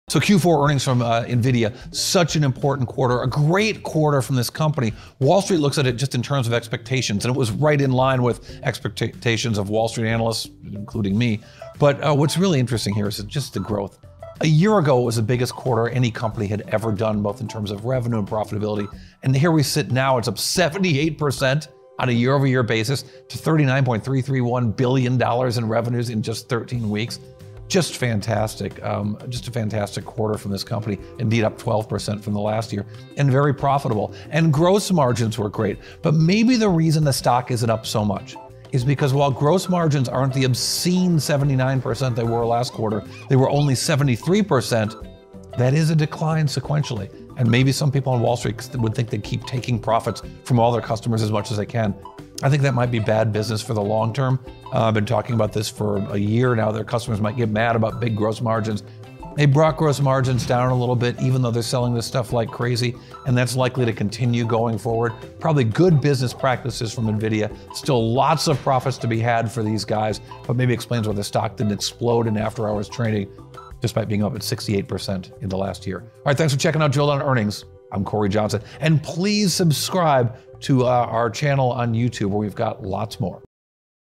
DDE Shorts 306: What you don’t know about NVIDIA Q4–deep dive into the technology & numbers ($NVDA)
🎙️ The Drill Down: NVIDIA Q4 Earnings Deep Dive

💡 Beyond the numbers: Unpack NVIDIA ($NVDA) Q4 performance with CEO Jensen Huang. More than “beat” or “miss” –the Drill Down Earnings with Epistrophy Capital Research’s chief market strategist Cory Johnson. It’s the business news behind stocks on the move.

🔍 In just 5 minutes, get:
Key financial highlights
Strategic moves with insight into the industry
Unfiltered information straight from NVIDIA leadership

🧠 Expert analysis by Cory Johnson, Chief Market Strategist at Epistrophy Capital Research
🚀 Catch snappy 1-minute highlights on YouTube Shorts, Instagram & TikTok!